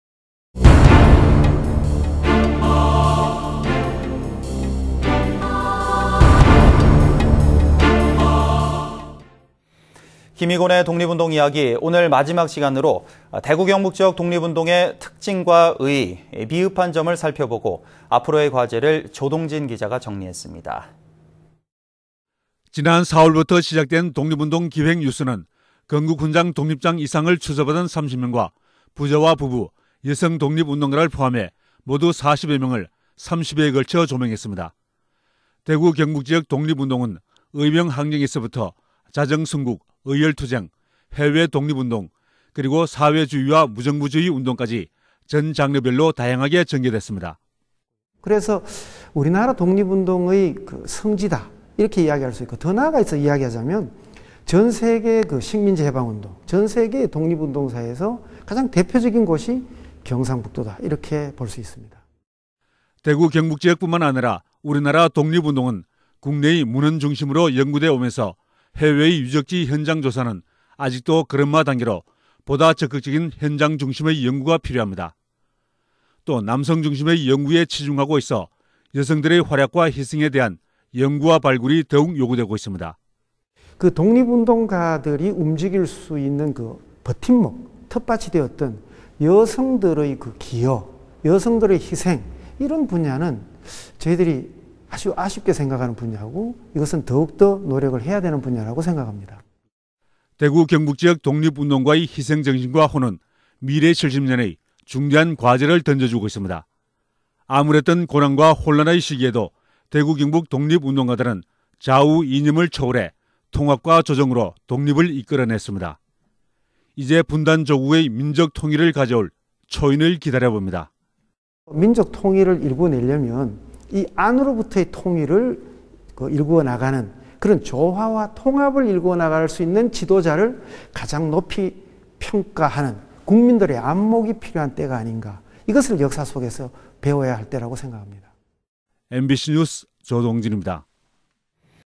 [안동MBC뉴스]김희곤의 독립운동이야기(31)..총정리
◀ＡＮＣ▶
광복 70주년을 맞아 대구경북지역 독립운동가를
탐방하는 김희곤의 독립운동이야기입니다.

오늘은 마지막으로 대구경북지역 독립운동의
특징과 의의,미흡한 점을 살펴보고
앞으로의 과제를 조동진기자가 정리했습니다.
◀ＥＮＤ▶

◀ＶＣＲ▶

지난 4월부터 시작된 독립운동 기획뉴스는
건국훈장 독립장 이상을 추서받은 30명과
부자와 부부,여성 독립운동가를 포함해
모두 40여명을 30회에 걸쳐 조명했습니다.

대구경북지역 독립운동은 의병항쟁에서부터
자정순국,의열투쟁,해외독립운동 그리고
사회주의와 무정부주의 운동까지
전 장르별로 다양하게 전개됐습니다.
◀ＩＮＴ▶
김희곤 교수 -경상북도독립운동기념관장-
" 그래서 우리나라 독립운동의 성지다 이렇게
이야기할 수 있고 더 나아가서 이야기하자면
전세계 식민지해방운동 전세계 독립운동사에서
가장 대표적인 곳이 경상북도로 이렇게 볼 수
있습니다."

대구경북지역 뿐만 아니라 우리나라 독립운동은
국내의 문헌중심으로 연구돼 오면서
해외의 유적지 현장조사는 아직도 걸음마
단계로 보다 적극적인 현장중심의 연구가
필요합니다.

또 남성중심의 연구에 치중하고 있어
여성들의 활약과 희생에 대한 연구와 발굴이
더욱 요구되고 있습니다.
◀ＩＮＴ▶
"독립운동가들이 움직일 수 있는 버팀목,텃밭이
되었던 여성들의 기여,여성들의 희생 이런
분야는 저희들이 아주 아쉽게 생각 하는 분야고
이것은 더욱 더 노력해야 하는 분야라고 생각합니다."

대구경북지역 독립운동가의 희생정신과 혼은
미래 70년의 중대한 과제를 던져주고 있습니다.

암울했던 고난과 혼란의 시기에도
대구경북 독립운동가들은 좌우 이념을 초월해
통합과 조정으로 독립을 이끌어 냈습니다.

이제 분단 조국의 민족통일을 가져올 초인을
기다려봅니다.
◀ＩＮＴ▶
"민족통일을 일구어내려면 안으로부터의 통일을
일구어 나가는 그런 조화와 통합을 일구어
나갈 수 있는 지도자를 가장 높이 평가하는
국민들의 안목이 필요할 때가 아닌가
이것을 역사속에서 배워야할 때라고 생각합니다."

mbc news 조동진입니다.